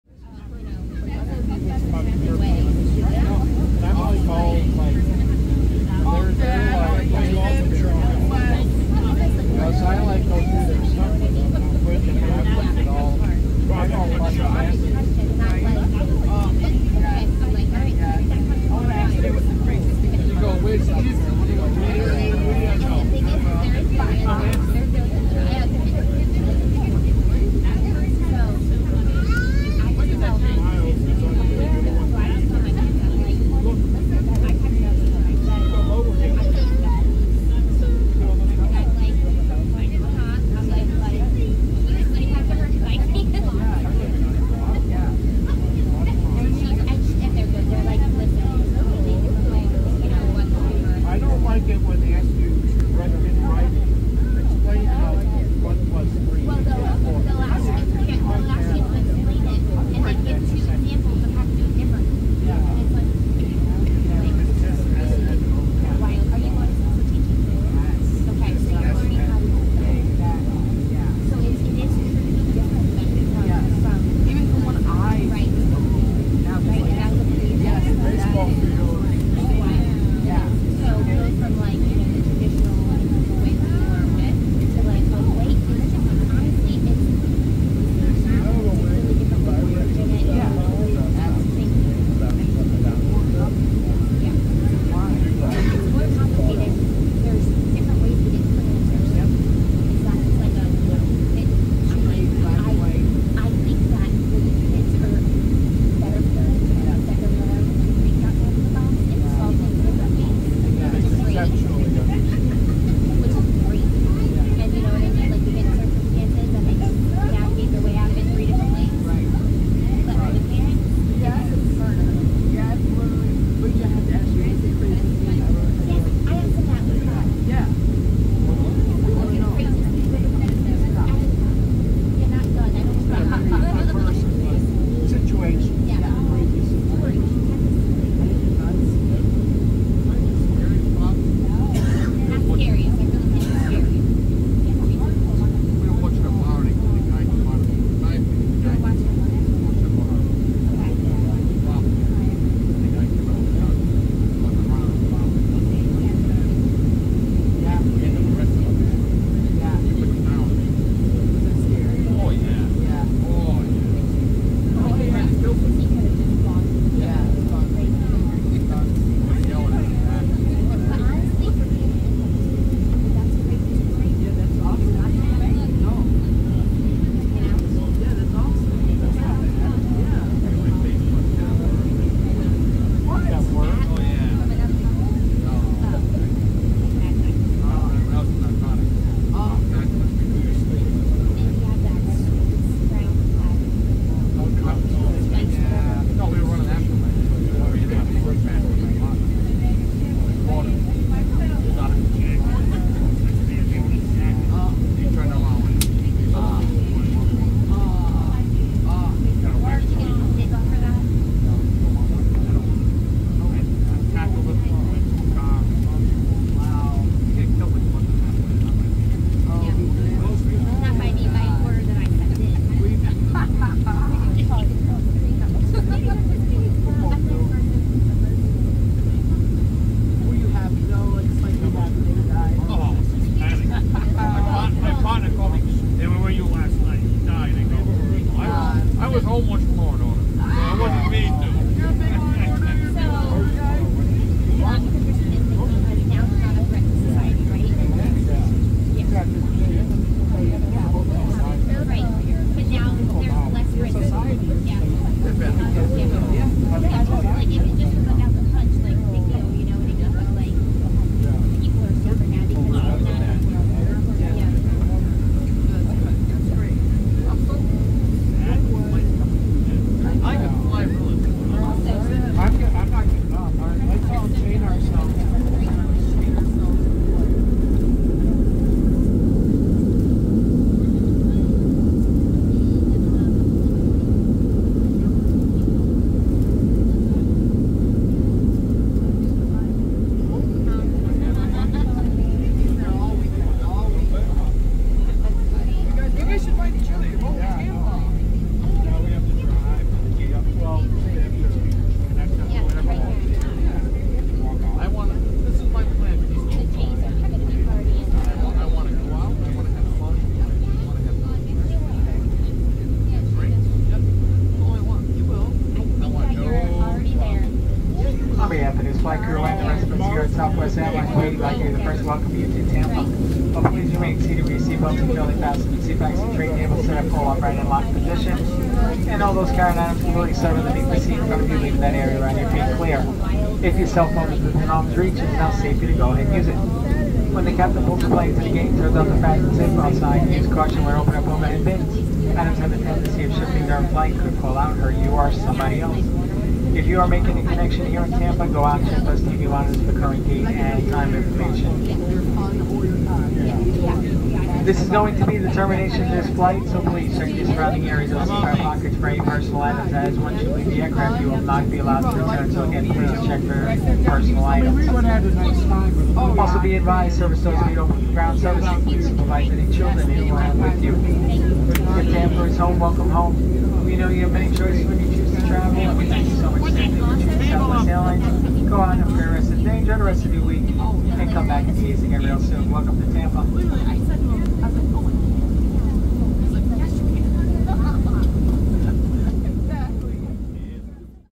Southwest Airlines Boeing 737-700 Landing - Tampa International Airport
Southwest Airlines arrives in sunny Florida.

Route:  Baltimore, MD (BWI) - Tampa, FL (TPA)
Flight Number: WN3454
Aircraft: Boeing 737-700
AC Registration: N288WN (16.2 Yrs Old)
Date: October 19th, 2023
Arrival Time: 2:49 PM EDT
Flight Time: 2h 02m
Seat: 23A
Distance: 890 Miles
Runway: 19L
Camera: Galaxy S23 Ultra